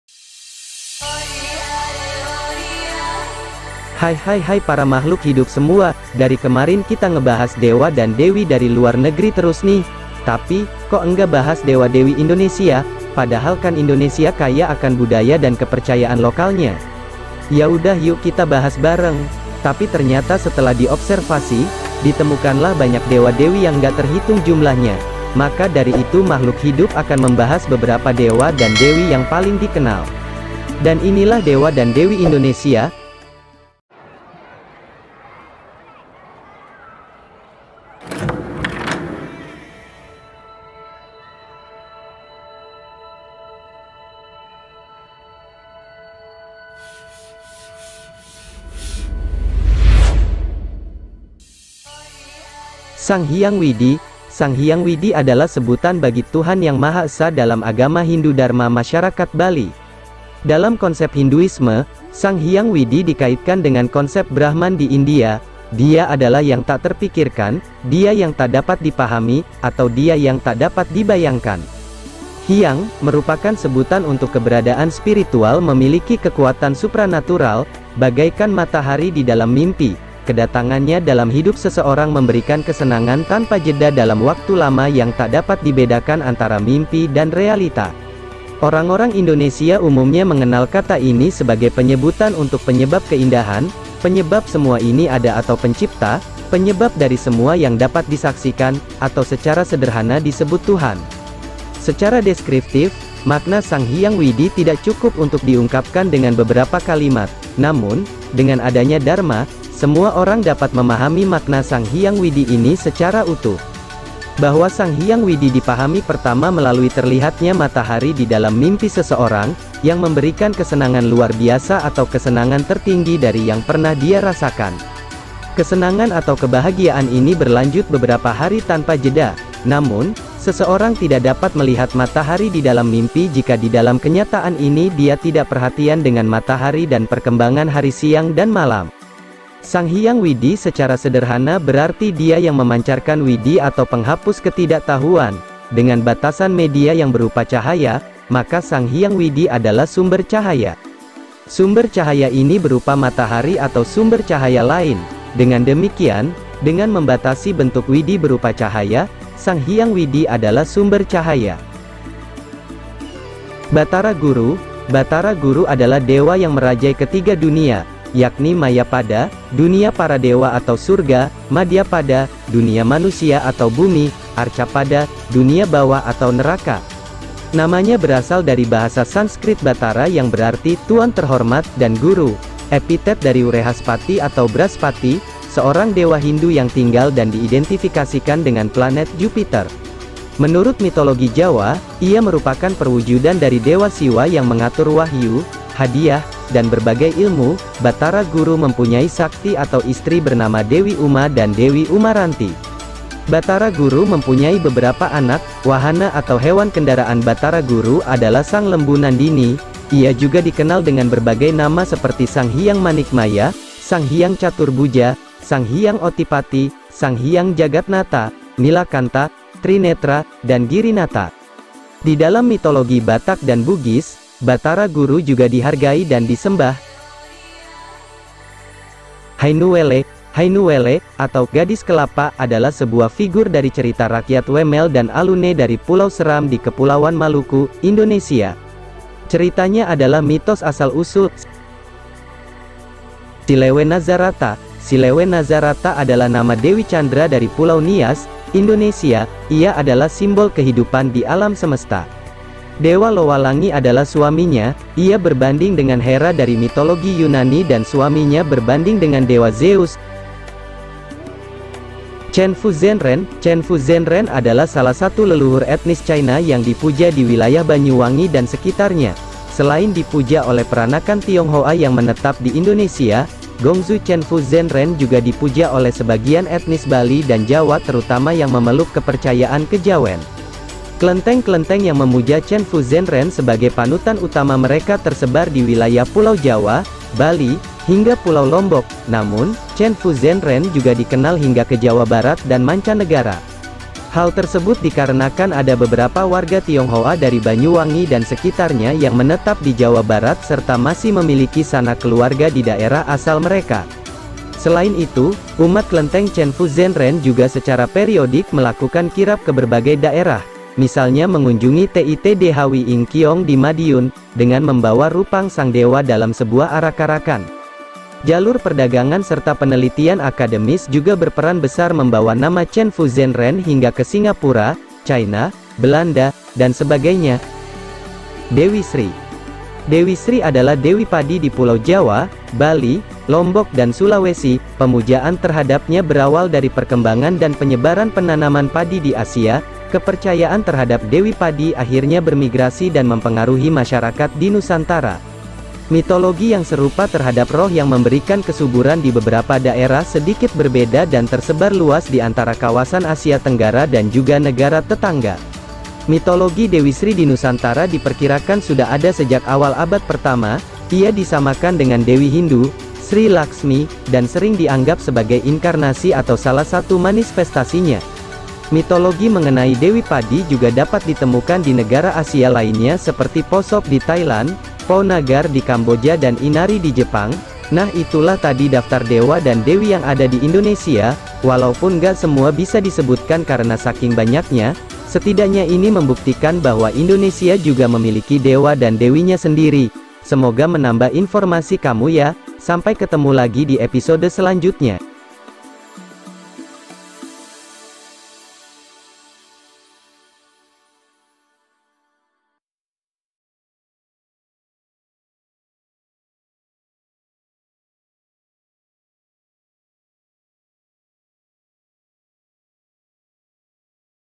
Indonesia Juga Punya Dewa dan Dewi loh! Nggak Kalah Keren!
Hai hai hai para makhluk hidup semua?
Dari kemarin kita ngebahas dewa dan dewi dari luar negri terus nih. Tapi, kok ngga bahas dewa dewi Indonesia? Padahal kan Indonesia kaya akan budaya dan kepercaaan lokalnya. Yaudah yuk kita bahas bareng, tapi ternyata setelah di observasi, ditemukanlah banyak dewa dewi yang nggak terhitung jumlahnya. Maka dari itu makhluk hidup akan membahas beberapa dewa dan dewi yang paling di kenal. Dan inilah dewa dan dewi Indonesia!